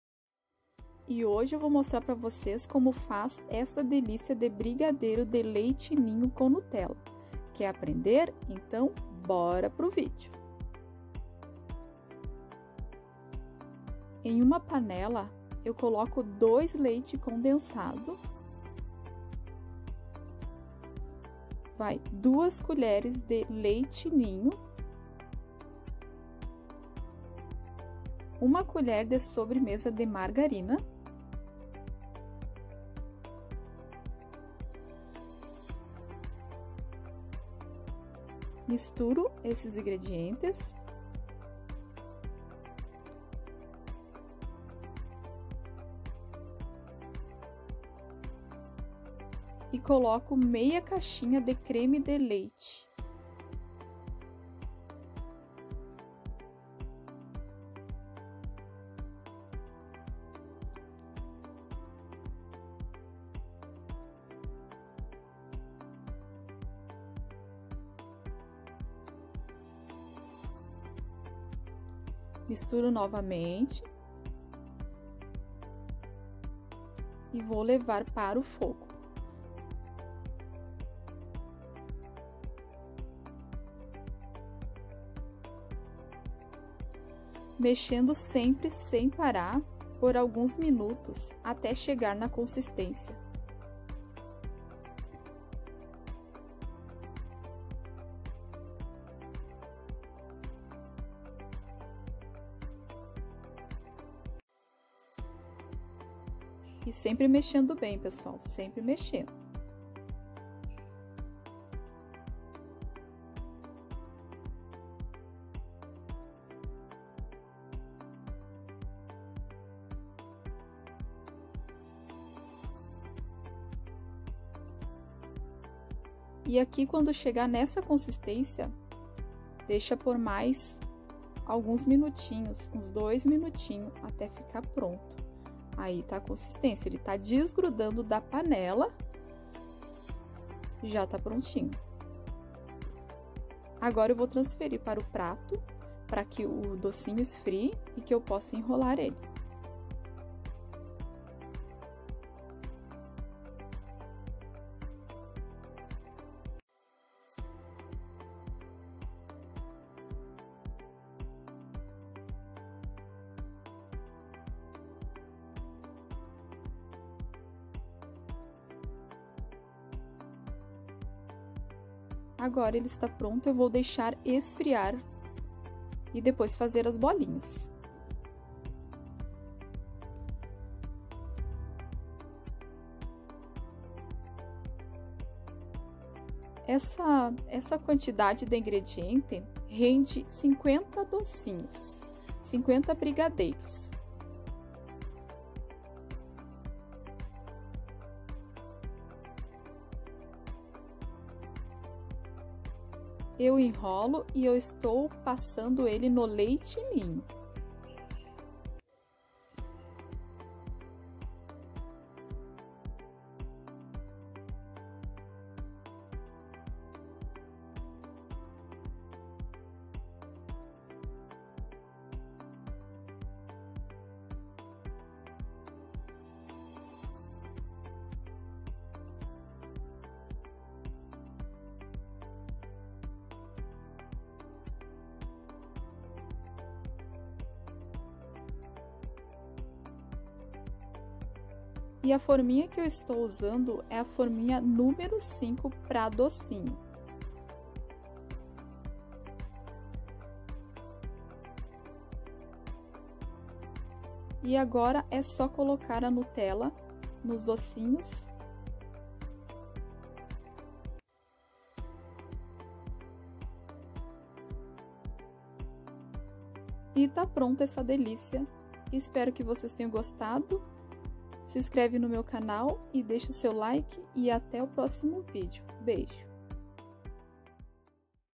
Brigadeiro de leite ninho com Nutella! simples e fácil!😍
Brigadeiro de leite ninho com Nutella;

2 caixas de leite condensado
2 colheres de sopa de leite ninho
1 colher de sobremesa de margarina
1/2 caixa creme de leite

Nutella